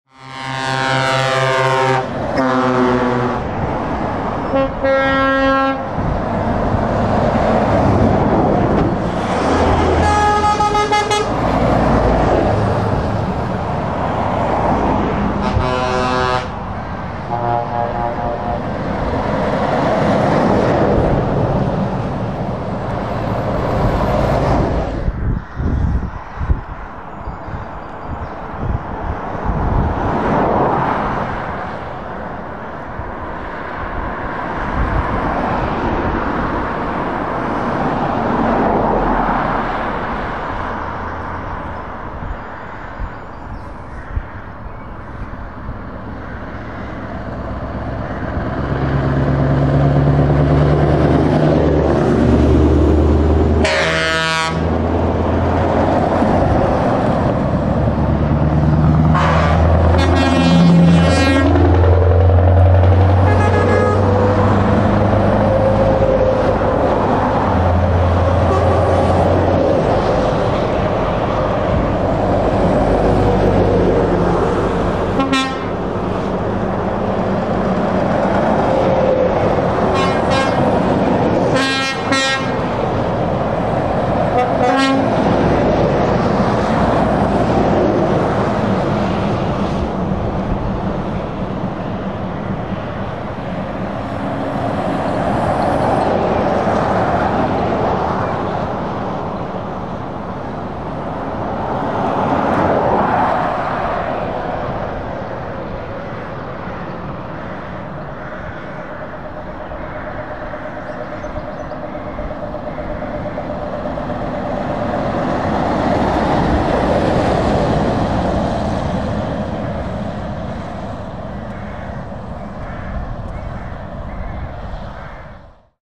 The best trucks come to the truck festival - TruckFest Lincoln
SCANIA SUPER R580 V8 - GWM Farms
DAF XF - Lyndas Transport
JKM Transport Solutions
SCANIA SUPER - Haddington
SCANIA - Brian Catchpole Transport
SCANIA - MALCOLM
VOLVO T-Cab - MALCOLM
VOLVO FH - MALCOLM
SCANIA R450 - MALCOLM
SCANIA G540 XT - MALCOLM
VOLVO FH750 - David Watson Specialist Transport
VOLVO FM - David Watson Specialist Lifting
DAF XG - MALCOLM
SCANIA SUPER R520 V8 - Jack Hannon